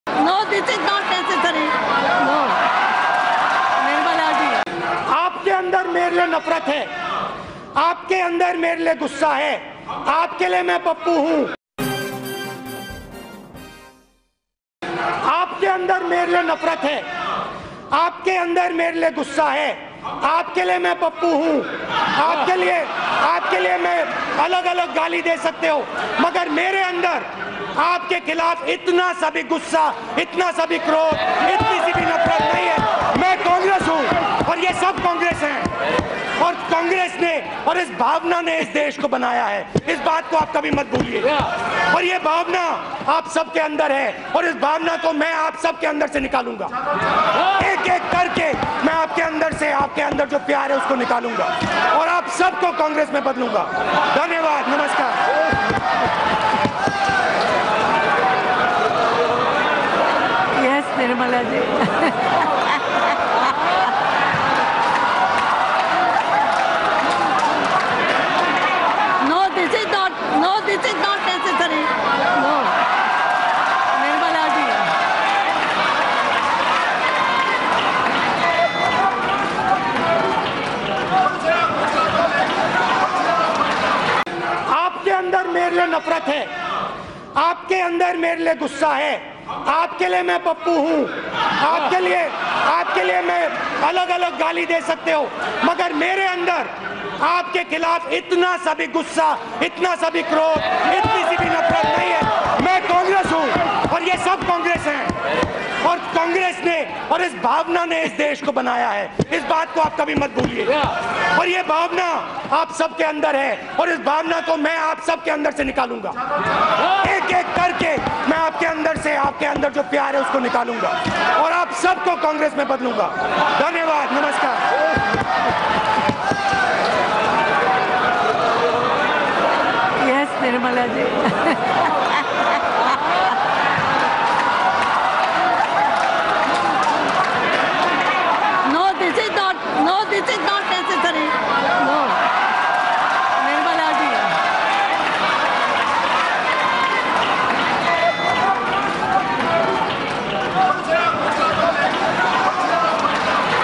No Confidence Motion: जब Rahul Gandhi ने PM Modi को गले लगाया था, खुद को पप्पू बोला था पूरा Video
No Confidence Motion: जब Rahul Gandhi ने PM Modi को गले लगाया था, खुद को पप्पू बोला था, देखें पूरा वीडियो.